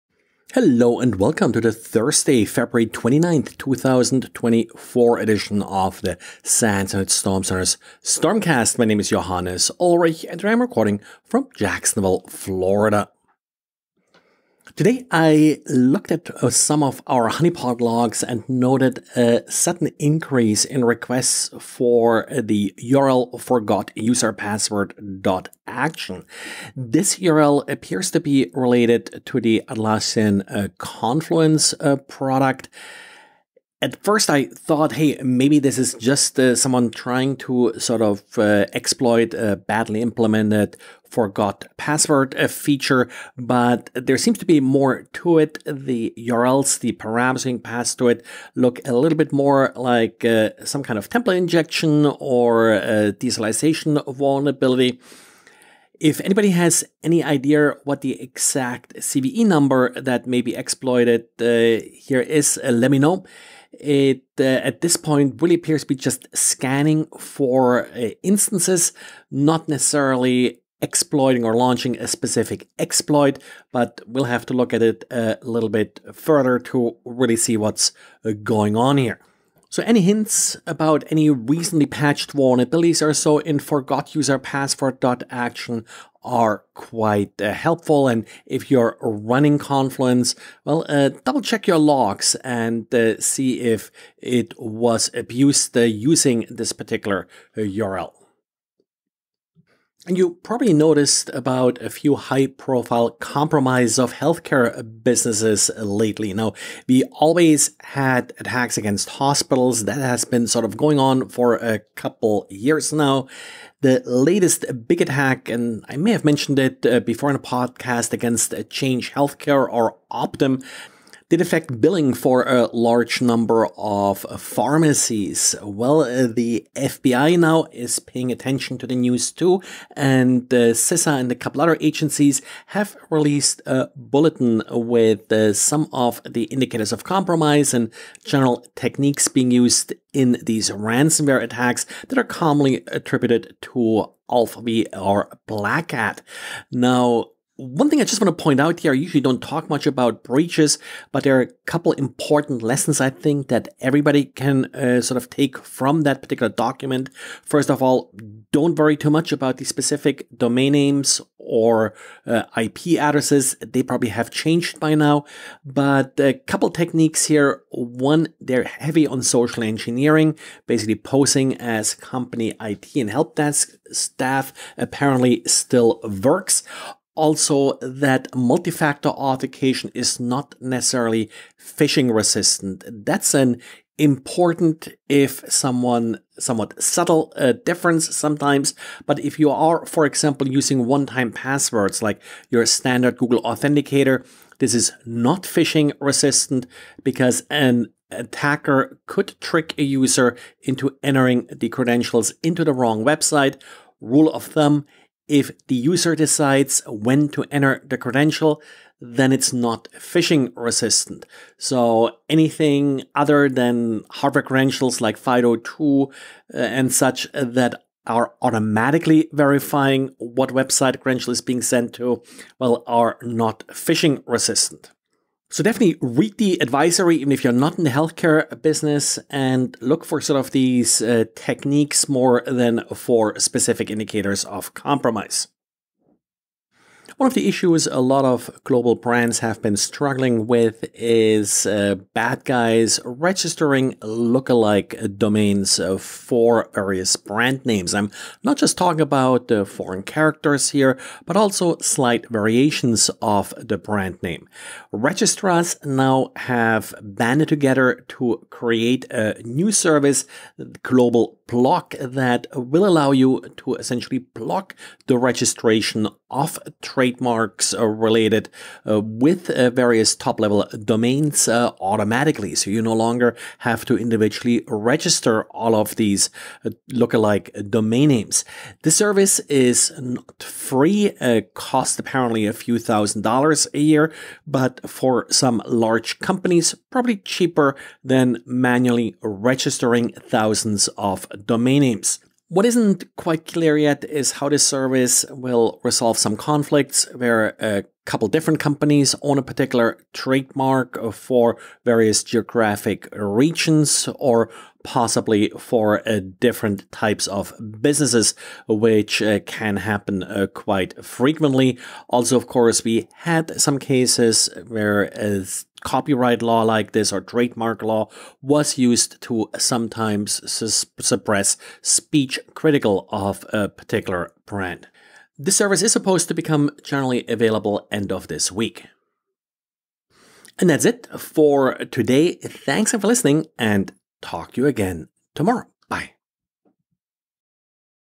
Network Security News Summary for Thursday February 29th, 2024
Odd Confluence Scan; ALPH/Blackcat Healthcare Attacks; GlobalBlock Released